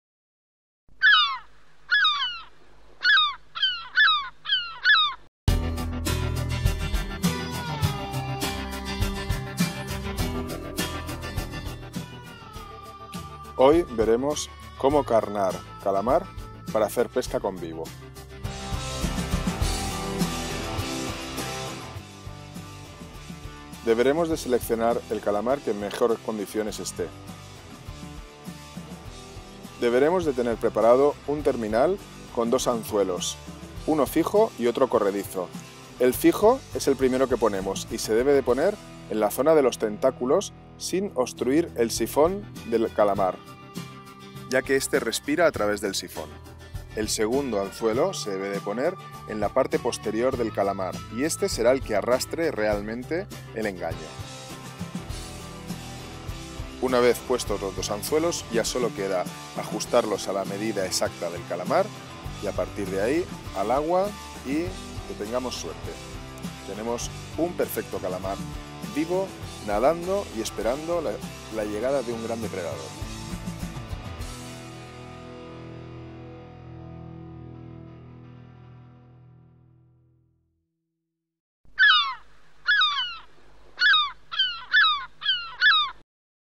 COMO CARNAR CALAMAR PARA PESCAR CON VIVO
video para el blog de pesca en el que se enseña como se debe carnar un calamar vivo para usarlo de crbo en la pesca de curricán. Atención este video puede herir la sensibilidad del espectador.